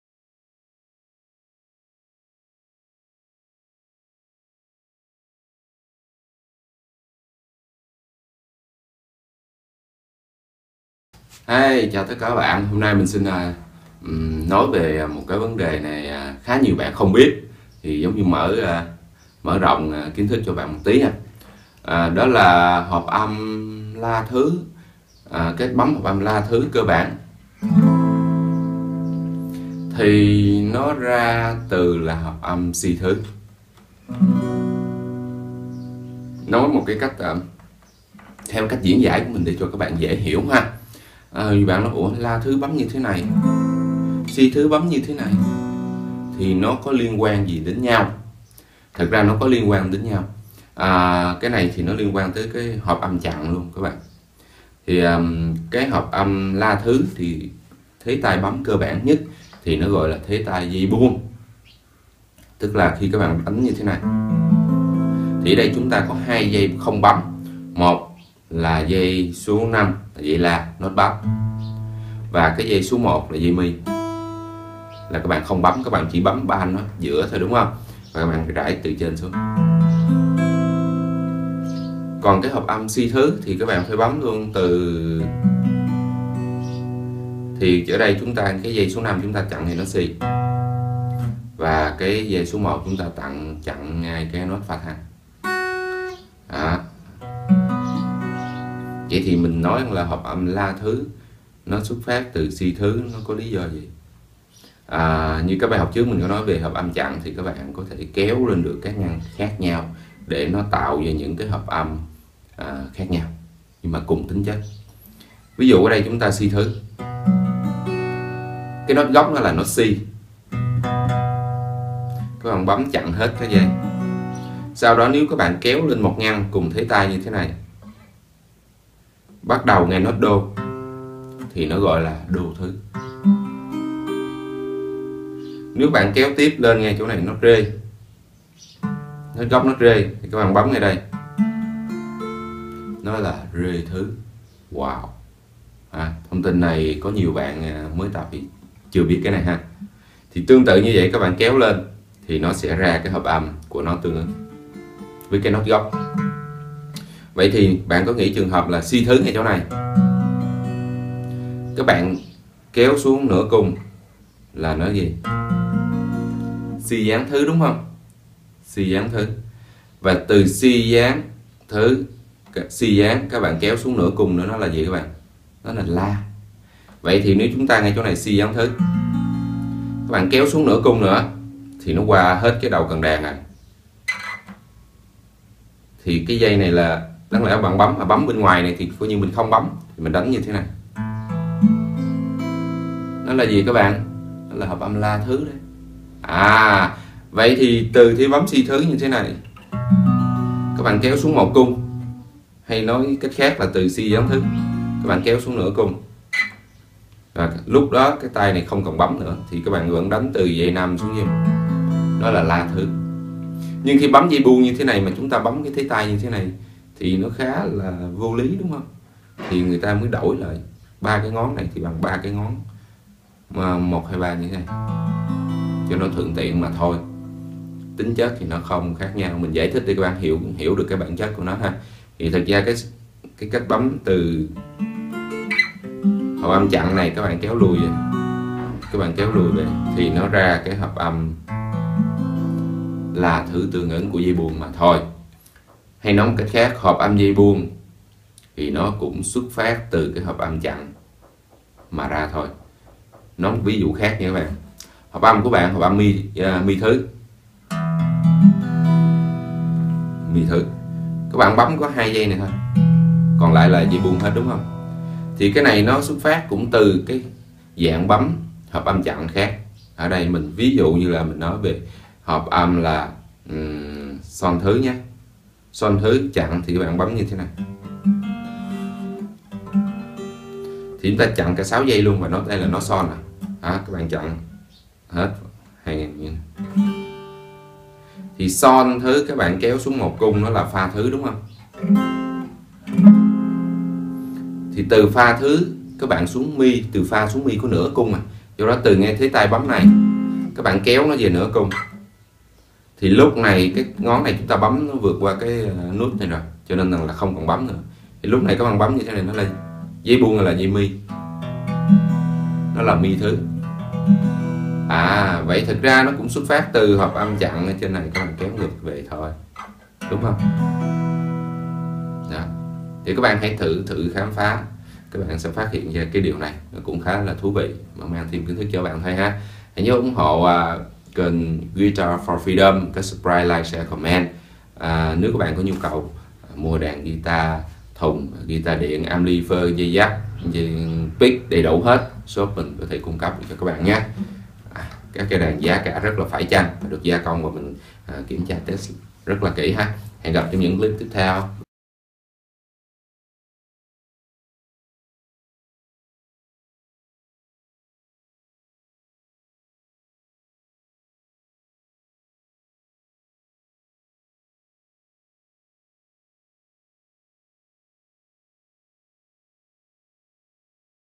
Giải thích tại sao thế bấm hợp âm Am từ Bm - Tìm hiểu hợp âm guitar cơ bản | Guitar4Freedom
Giải thích tại sao thế bấm  Am từ Bm - Tìm hiểu hợp âm guitar cơ bản | Guitar4Freedom